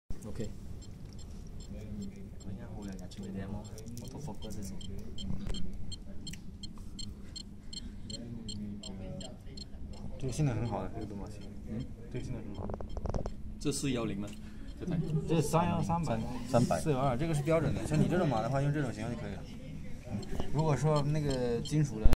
Multi barcode reading with datalogic matrix 300
You can capture up to 200 barcode in single scan with datalogic matrix 300 2D barcode scanner. Visit below site for more matrix 300 information: